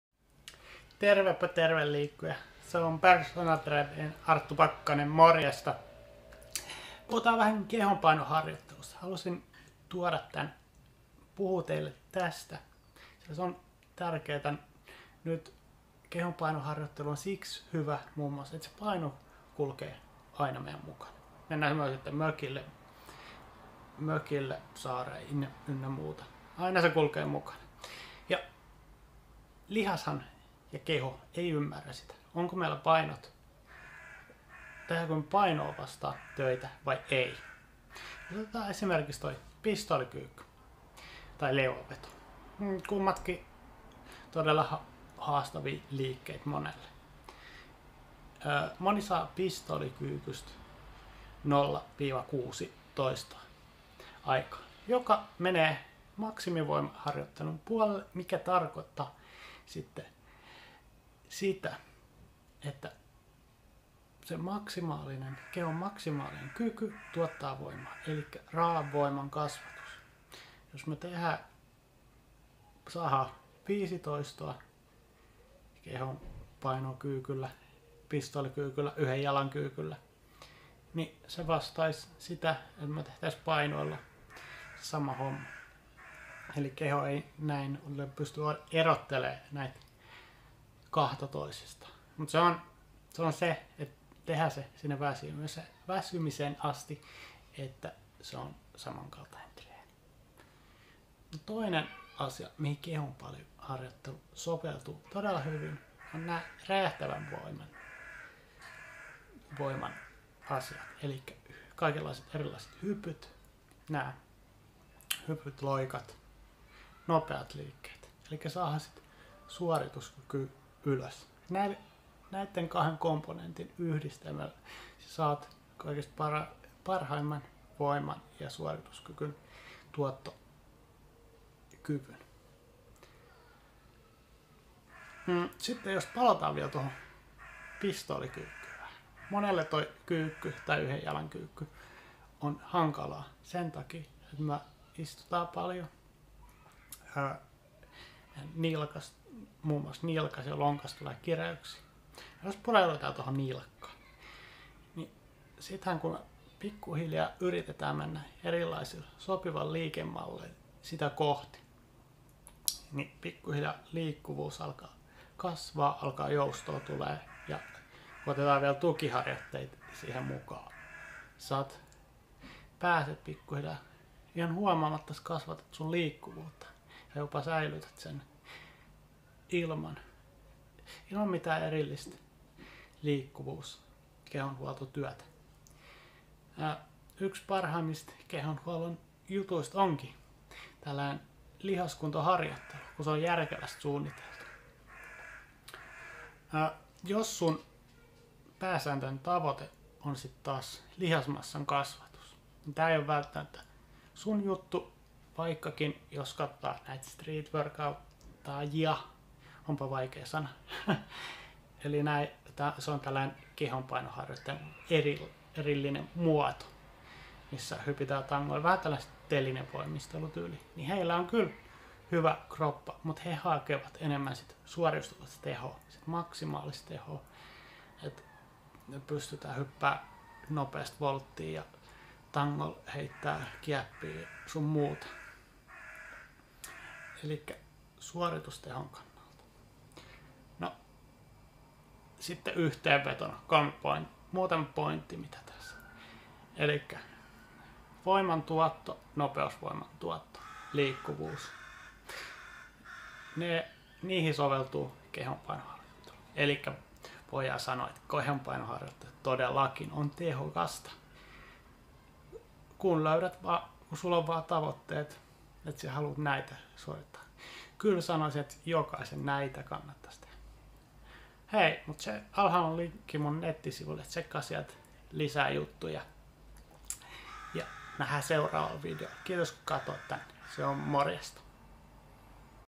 Onko kehonpainoharjoittelu tehokasta
Moni ajattelee että kehonpaino harjoittelu on tehotonta ... näin se on vain silloin kun ei tiedetä mitä tehdään ja harjoittelu on järjetöntä katso video ja opi lisää.

Hei liikkuja , Personal Trainer Arttu Pakkanen moi puhutaan vähän kehonpaino harjoittelusta se on hyvä liikuntamuoto mm. siksi että paino kulkee meillä aina mukana halusimme tai emme.

Lihashan ja keho ei ymmärrä tehdäänkö me painolla ja ilman painoa otetaan esimerkiksi pistoolikyykky tai leuanveto.

Kummatkii todella haastavii liikkeitä monelle moni saa pistoolikyykystä eli 1 jalan kyykystä 0 - 6 toistoa aikaan joka menee maksimi voima harjoittelun puolelle joka tarkoittaa kehon maksimaalista kykyä tuottaa voimaa eli raakaa voimaa jos saadaan esim. 5 toistoa pistoolikyykyllä kehonpainokyykyllä niin se vastaisi samaa asiaa paino liikkeellä.

Eli keho ei näin pysty erottelemaan näitä toisistaan mutta liike pitää tehdä sinne väsymykseen asti jotta treeni on tehokas.

Toinen asia missä kehopaino harjoittelu menee muista muodoista ohi on räjähtävän voiman harjoitteet eli kaikenlaiset hypyt , loikat noepat liikkeet eli parannetaan suoritustehoa kun yhdistät sekä maksimi voimaa että nopeus / räjähtävää voimaa saat parhaan mahdollisen suoritustehon.

Palataan pistoliikyykkyyn monelle yhden jalan kyykky on hankalaa sentakii että istutaan paljon nilkka, lonkka kiristyy joka aiheuttaa erinnäisiä vaivoja jos ei hoideta kun treenit kovenee.

Pureudutaan vielä vähän nilkkaan jos yritetään pikkuhiljaa erinäkösillä liikemalleilla ja tukiharjoittella mennä sitä kohti liikkuvuus kasvaa , joustoo tulee pääset pitemmälle ilman erillistä liikkuvuus harjoittelua lihaskuntoharjoittelu onkin yksi parhaista kehoa huoltavista harjoitteista kun se on järkevästi suunniteltu.

Jos sun pääsääntönen tavoite on lihasmassan kasvatus tää ei oo sun juttu vaikka jos katsomme streetworkattaajia onpa vaikee sana :)  joka on eräänlainen kehonpainoharjoittelun muoto . Kyllä noillakin jannuilla kovia kroppia näkee vaikkakin heiläkin pääsääntönen tavoite on suorituskyvyn parantaminen.

TOP 3 pointit
- voiman tuotto
- nopeusvoiman tuotto
-liikkuvuus
Eli voidaan sanoa että kehonpainoharjoittelu todellakin on tehokasta kun sulla on vain tavoitteet ja järki mukana ja haluat näitä asioita sanoisin että jokaisen näitä kannattais tehä . Alla linkki mun nettisivulle käy siellä tsekkailees asioita ja nähää seuraaval videol se on morjensta

Yhteystiedot:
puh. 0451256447